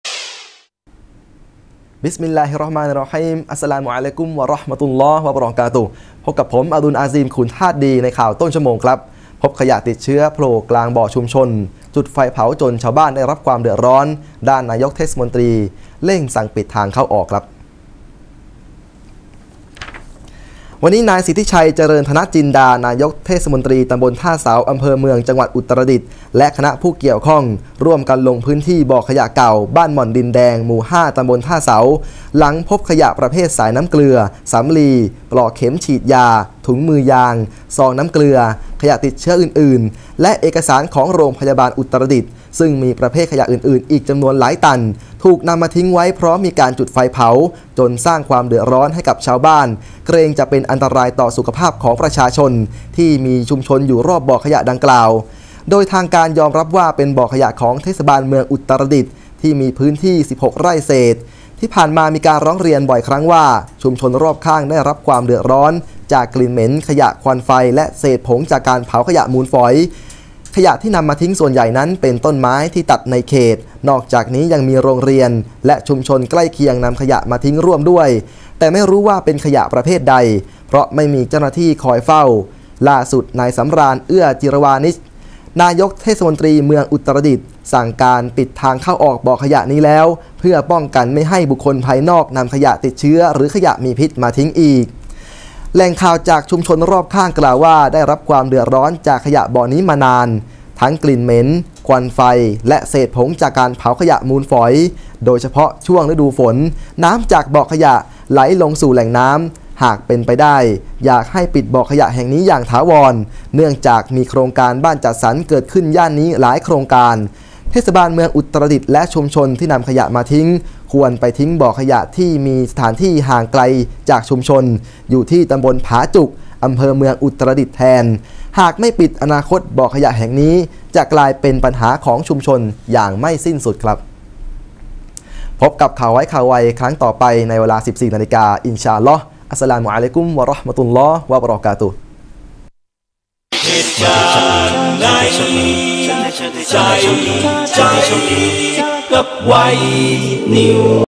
อุตรดิตถ์พบขยะติดเชื้อ-อุปกรณ์การแพทย์ถูกทิ้งบ่อขยะร้าง
วันนี้ นายสิทธิชัย เจริญธนะจินดา นายกเทศมนตรีตำบลท่าเสา อ.เมือง จ.อุตรดิตถ์
และคณะผู้เกี่ยวข้อง ร่วมกันลงพื้นที่บ่อขยะเก่า บ้านม่อนดินแดง หมู่ 5 ต.ท่าเสา หลังพบขยะประเภทสายน้ำเกลือ สำลี ปลอกเข็มฉีดยา ถุงมือยาง ซองน้ำเกลือ ขยะติดเชื้ออื่นๆ และเอกสารของโรงพยาบาลอุตรดิตถ์ ขยะประเภทอื่น ๆ อีกจำนวนหลายตัน ถูกนำมาทิ้งไว้พร้อมมีการจุดไฟเผา จนสร้างความเดือดร้อนให้กับชาวบ้าน เกรงจะเป็นอันตรายต่อสุขภาพของประชาชน ในชุมชนที่อยู่รอบบ่อขยะดังกล่าว
       โดยทางการ ยอมรับว่าเป็นบ่อขยะของเทศบาลเมืองอุตรดิตถ์ มีพื้นที่ 16 ไร่เศษ ที่ผ่านมามีการร้องเรียนบ่อครั้งว่า ชุมชนรอบข้างได้รับความเดือดร้อน จากกลิ่นเหม็นขยะ ควันไฟ และเศษผงจากการเผาขยะมูลฝอย ขยะที่นำมาทิ้งนั้นส่วนใหญ่เป็นต้นไม้ที่ตัดในเขต นอกจากนี้ยังมีโรงเรียน และชุมชนใกล้เคียงนำขยะมาทิ้งร่วมด้วย แต่ไม่รู้ว่าเป็นขยะประเภทใด เพราะไม่มีเจ้าหน้าที่คอยเฝ้า ล่าสุดนายสำราญ เอื้อจิรวาณิชย์ นายกเทศมนตรีเมืองอุตรดิตถ์ สั่งการให้ปิดทางเข้าออกบ่อขยะนี้แล้ว เพื่อป้องกันไม่ให้บุคคลภายนอกนำขยะติดเชื้อหรือขยะพิษมาทิ้ง
        แหล่งข่าวจากชุมชนรอบข้าง กล่าวว่า ได้รับความเดือดร้อนจากบ่อขยะมานานแล้ว ทั้งกลิ่นเหม็น ควันไฟและเศษผงจากการเผาขยะมูลฝอย โดยเฉพาะช่วงฤดูฝน น้ำจากบ่อขยะไหลลงสู่แหล่งน้ำ หากเป็นไปได้อยากให้ปิดบ่อขยะแห่งนี้อย่างถาวร เนื่องจากมีโครงการบ้านจัดสรรเกิดขึ้นย่านนี้หลายโครงการ เทศบาลเมืองอุตรดิตถ์และชุมชนที่นำขยะมาทิ้ง ควรนำไปทิ้งบ่อขยะที่มีสถานที่ที่ห่างจากชุมชนอยู่แล้วที่ ต.ผาจุก อ.เมืองอุตรดิตถ์แทน หากไม่ปิดอนาคตบ่อขยะแห่งนี้จะกลายเป็นปัญหาของชุมชนต่อไปไม่สิ้นสุด